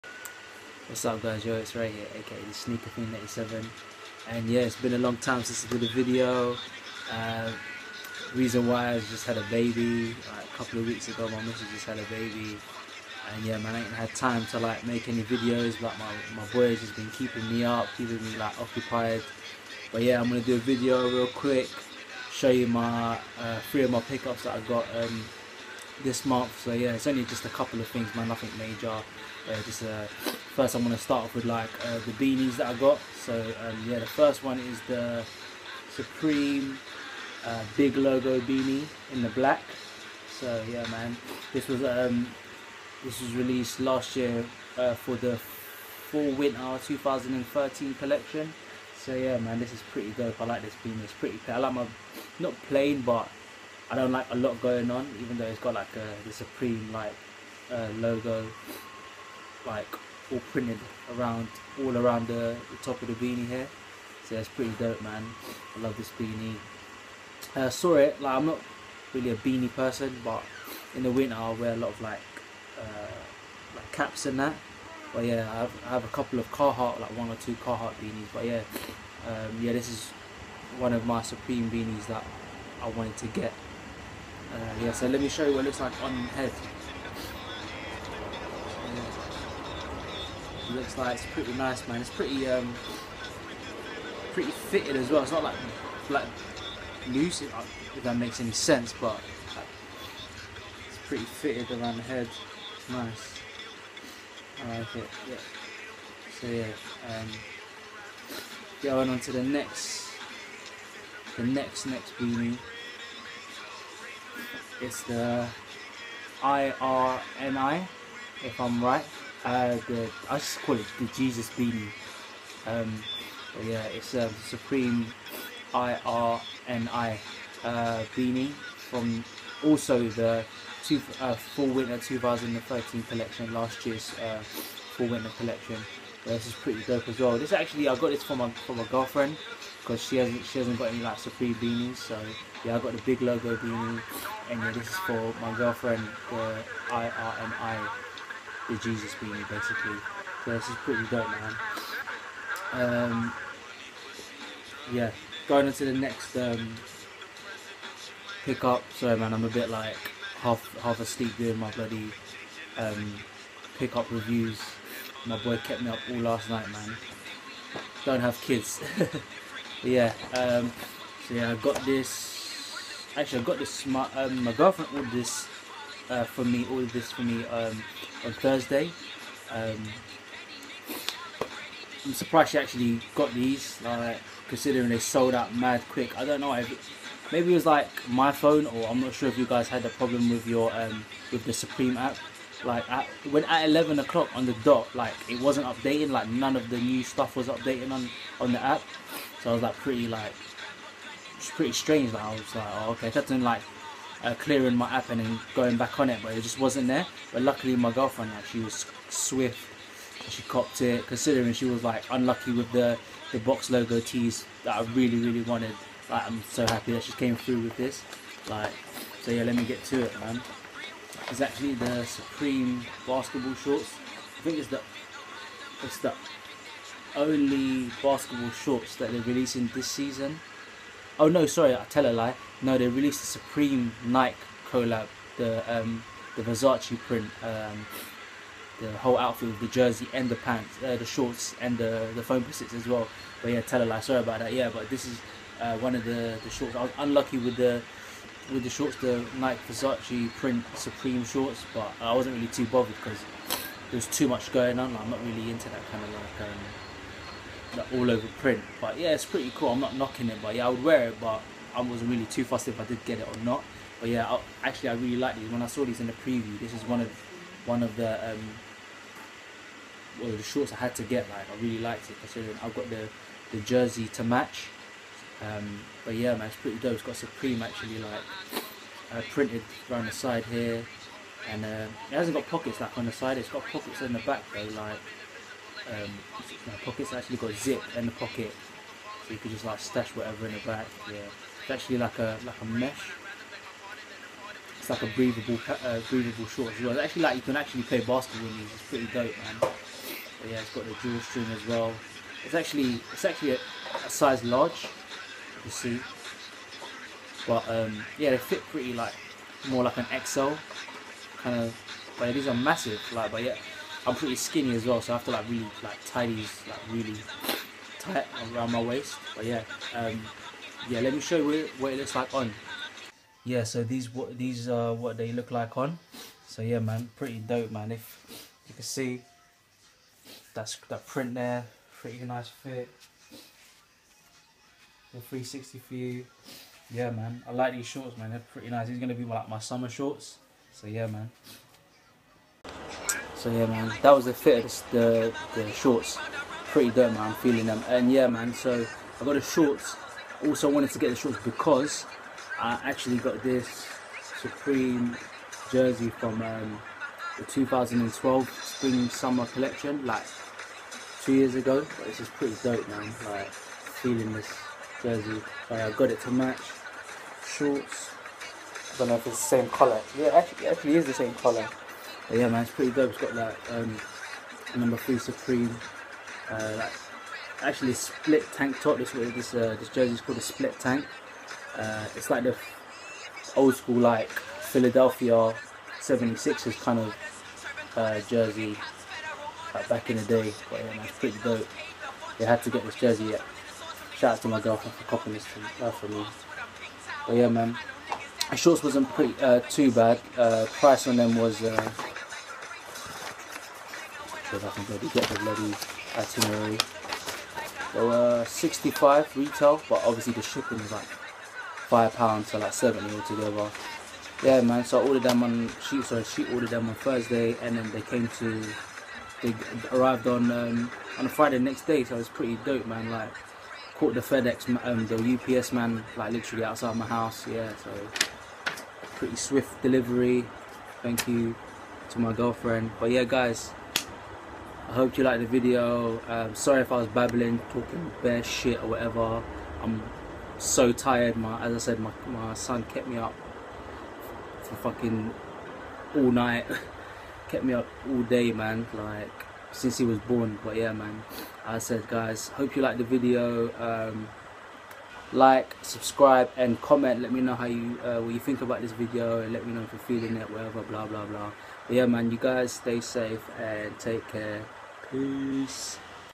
Latest Pick Up's Episode 4-Supreme Big Logo Beanie,Supreme INRI Beanie,Supreme Basketball Shorts.
My latest pick ups 01/06/14 hope you guys like the vid, apologies if I'm sniffing like mad my hayfever was killin me. -___-

Enjoy guys. Peace